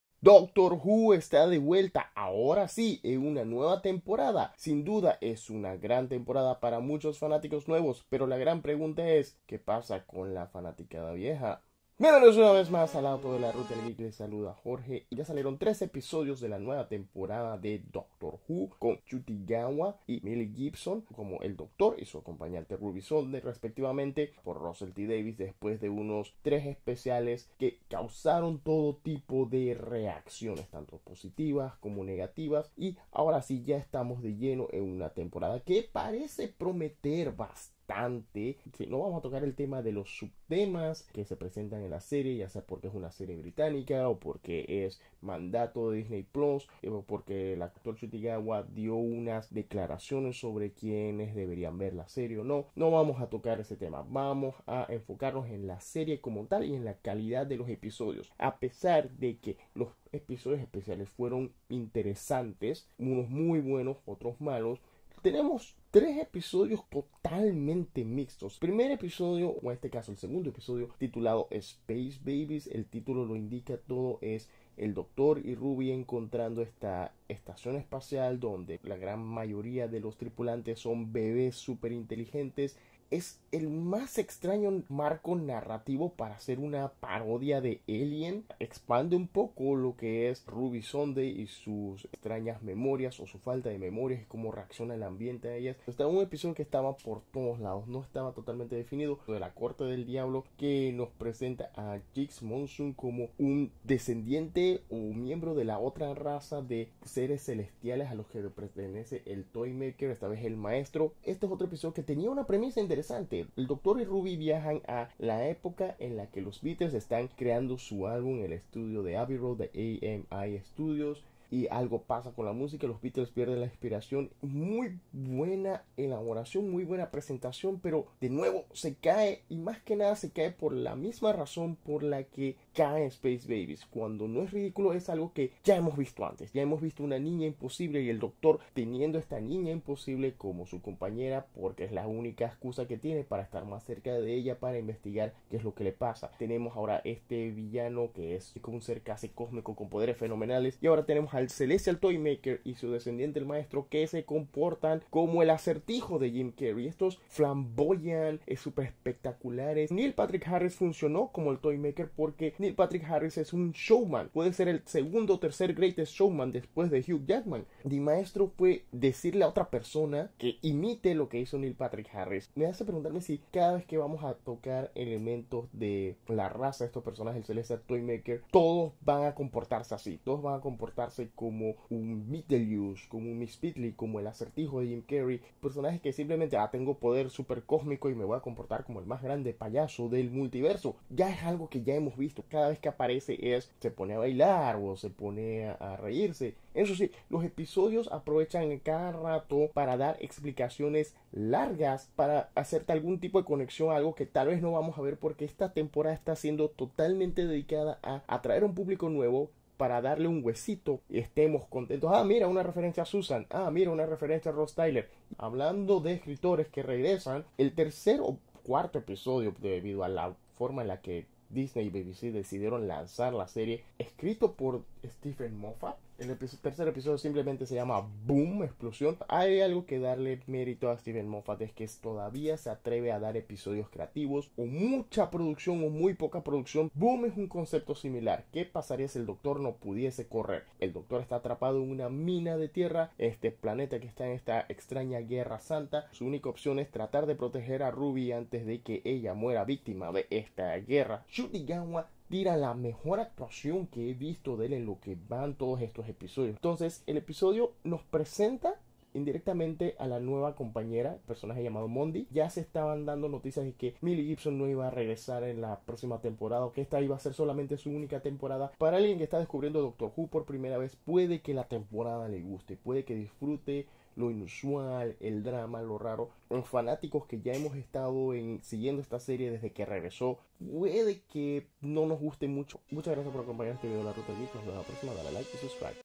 Doctor Who Episodios 2-4 | Crítica y Opinión
Doctor Who ha regresado... otra vez... pero será solamente para nuevos fanáticos o será una repetición con actores nuevos.